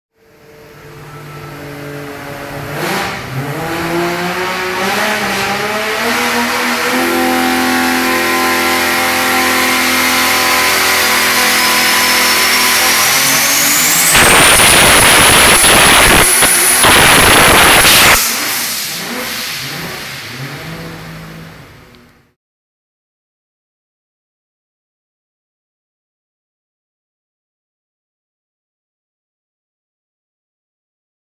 Gas Motorsport 3.2L 2JZ 2041HP Dyno Video
Here is the video of our dyno run when tuning the 3.2L engine.